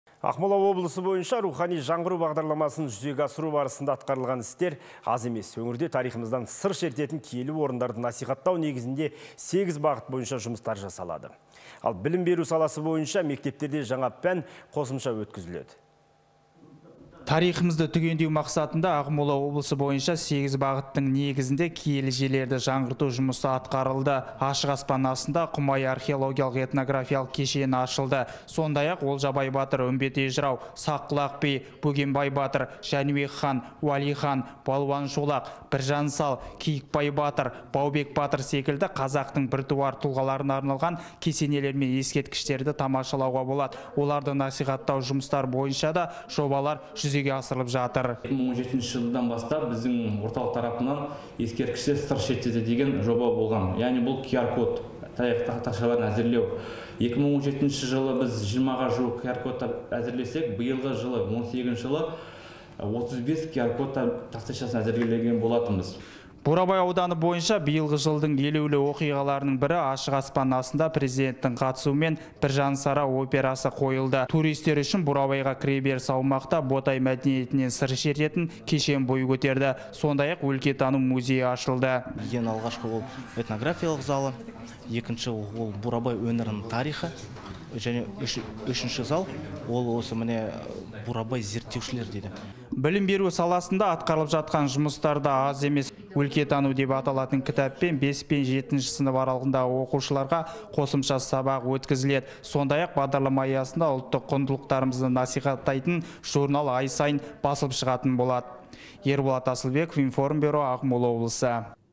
«Рухани жаңғыру» бағдарламасын жүзеге асыруда Ақмола облысының еңбегі аз емес
Ақмола облысы бойынша «Рухани жаңғыру» бағдарламасын жүзеге асыру барысында атқарылған істер аз емес. Өңірде тарихымыздан сыр шертетін киелі орындарды насихаттау негізінде сегіз бағыт бойынша жұмыстар жасалады. Ал білім беру саласы бойынша мектептерде жаңа пән қосымша өткізіледі.